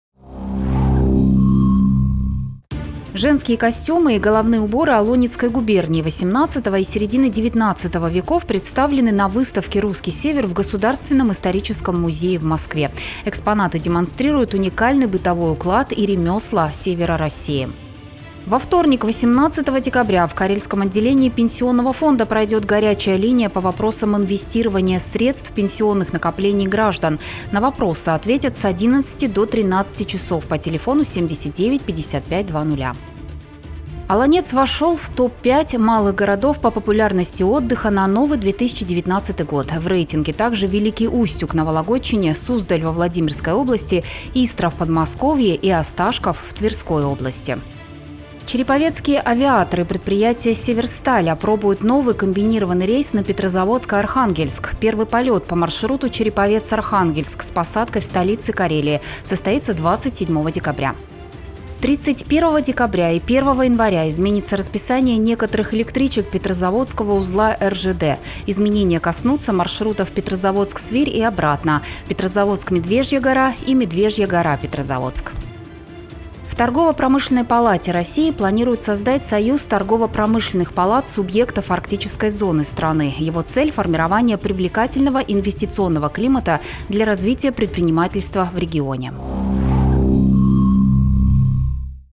Коротко 17.12.2018
Русский Север прописался в историческом музее, инвестиционный климат Арктики улучшат, Олонец будет самым популярным в новогодние праздники, электрички и самолеты немного поменяют маршруты. Обо всем этом расскажем коротко.